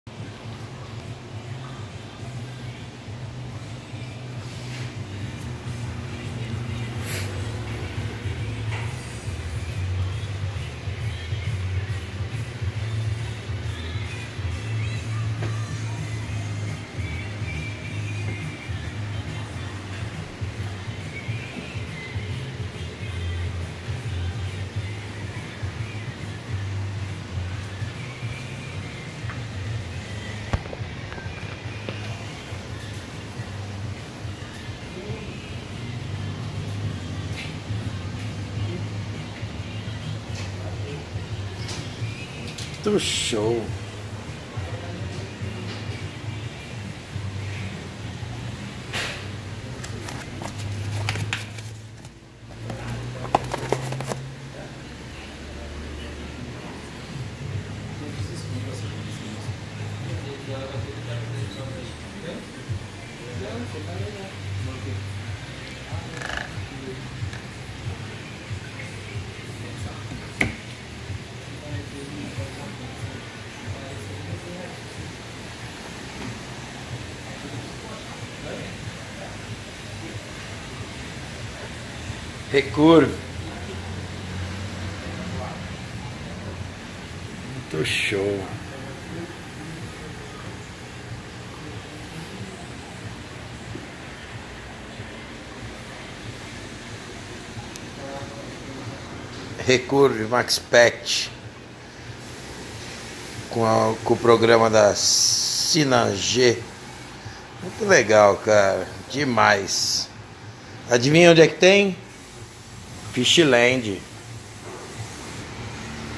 Chegada da Maxspect Recurve na Fishland.
Maxspect Recurve está à disposição para todos clientes da Fishland. Luminária top do mercado internacional  pronta para seu aquário.